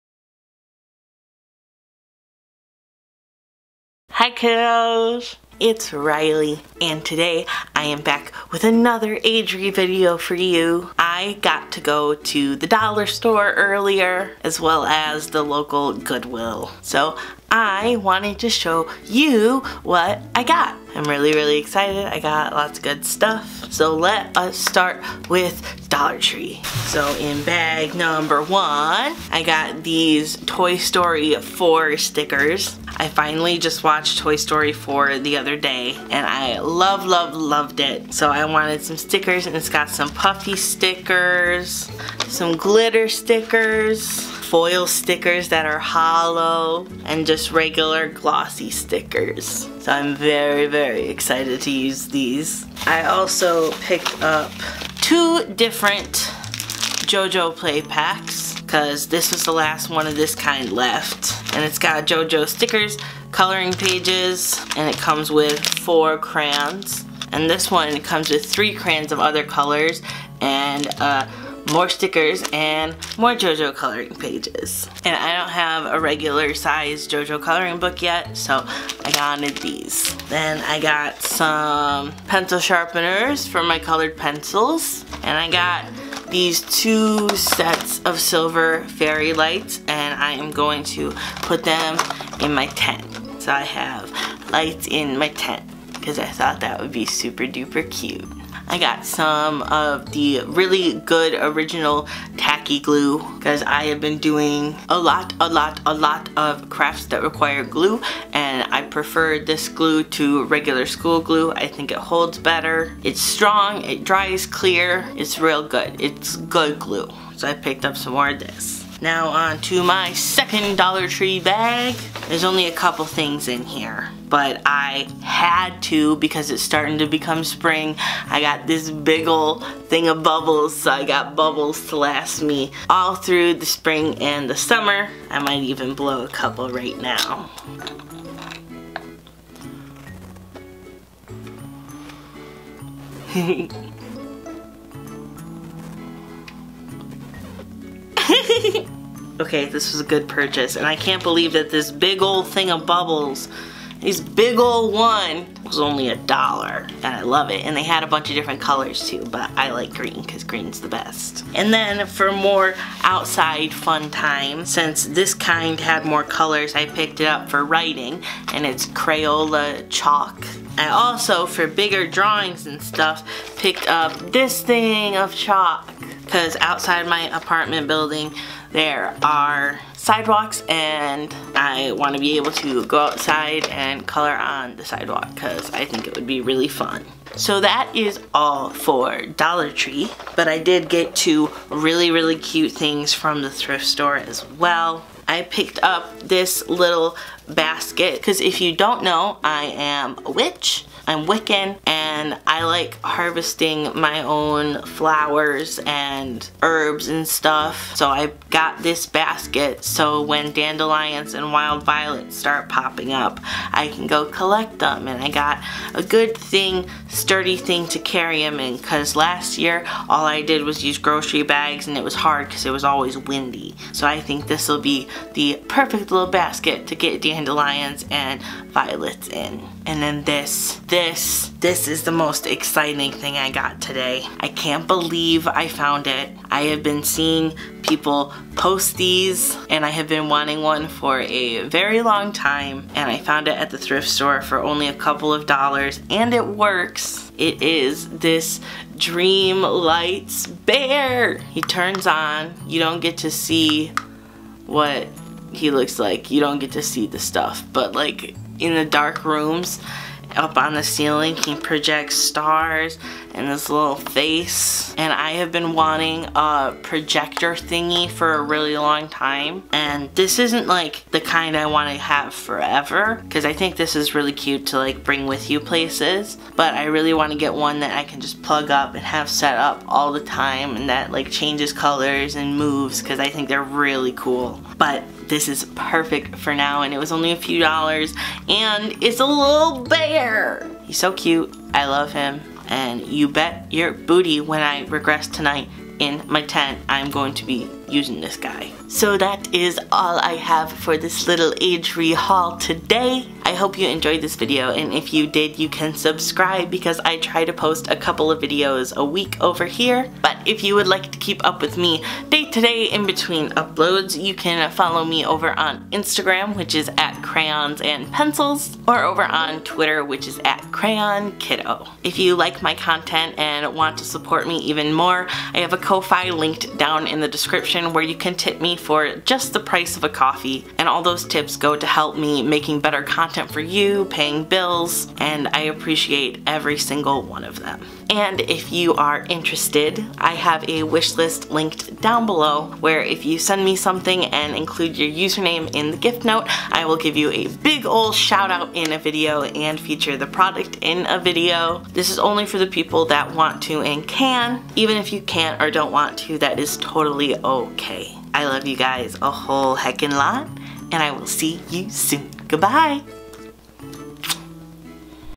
☆ a lil agere haul! (dollar tree + goodwill) ☆
☺︎ a teeny tiny haul 4 u!

quidd - yikesriley